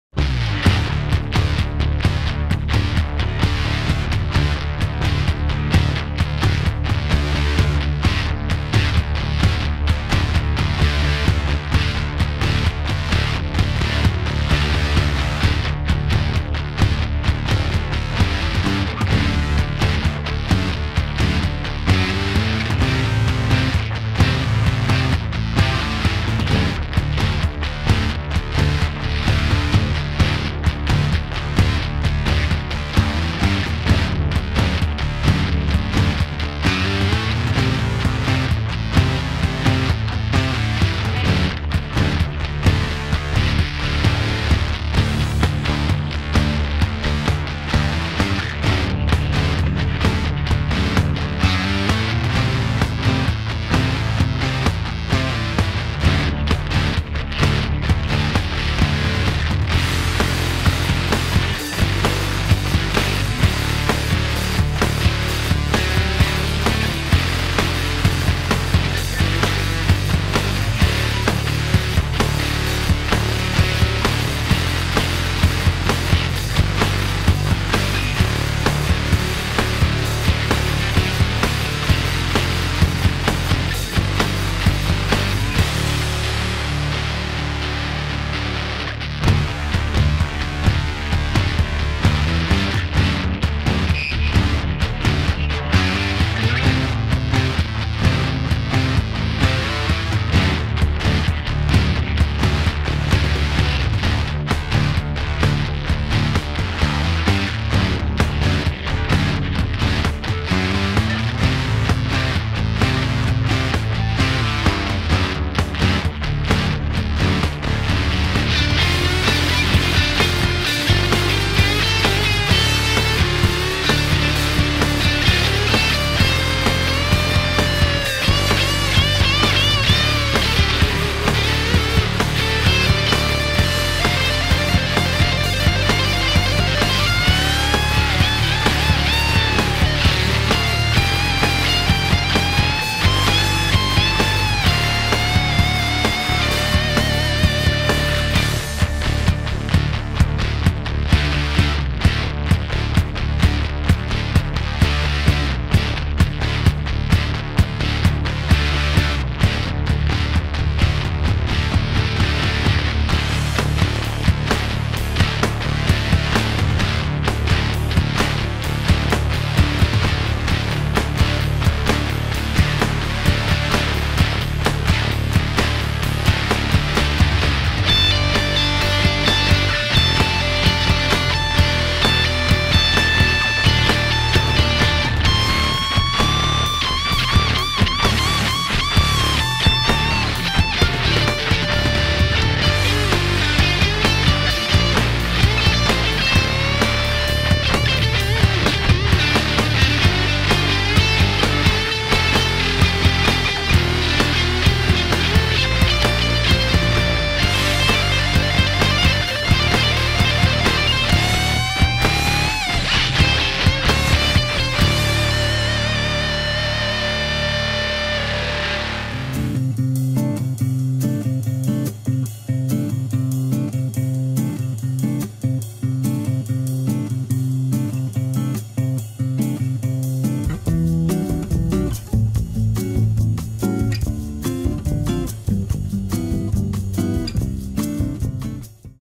Волейбол. Нападающий удар. Продолжение
Волейбол. Нападающий удар. Продолжение.  Суперлига. Россия. Ярославль. 17 февраля 2018.